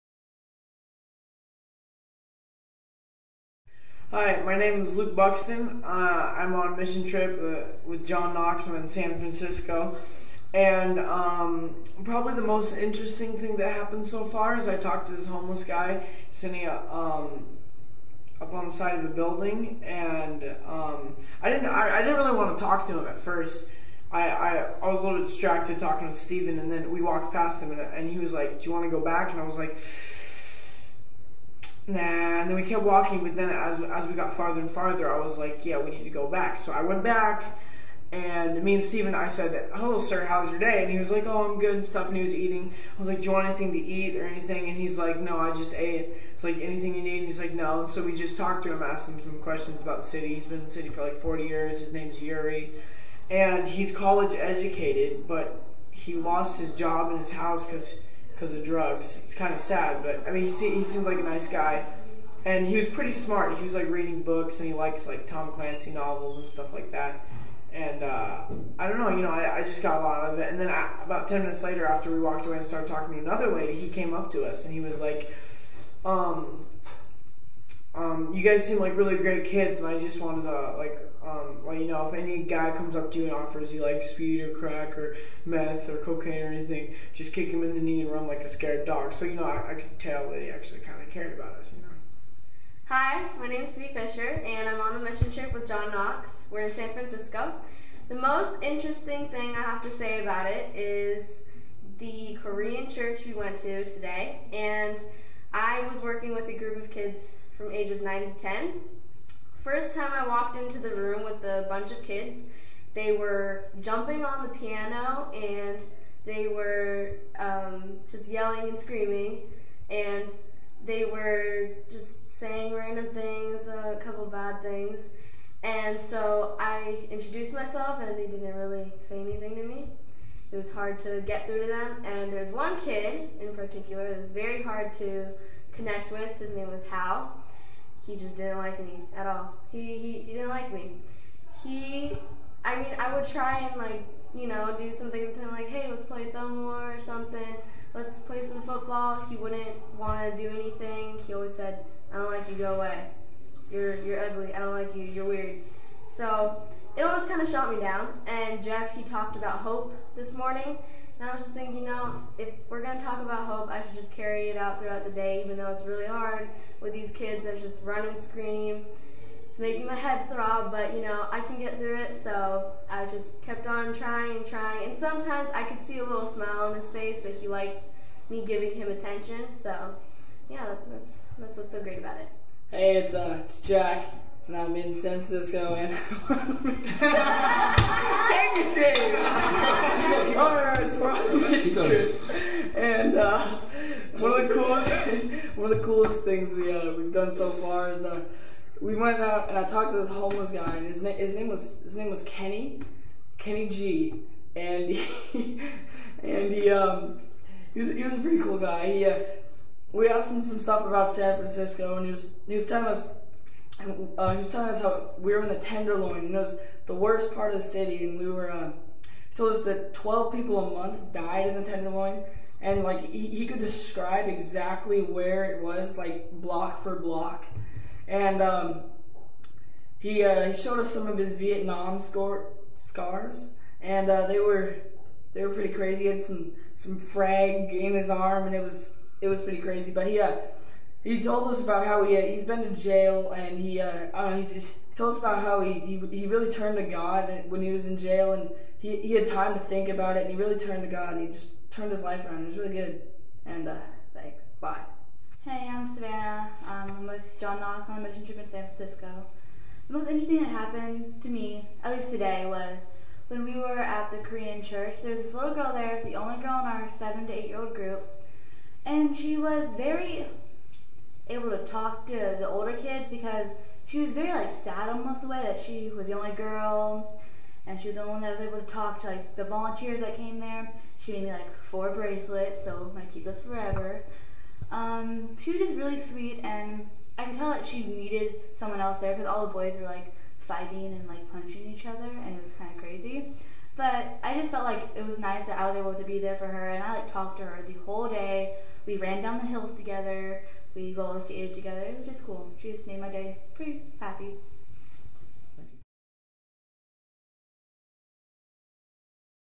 a word from our students
a few students share from their experiences in San Francisco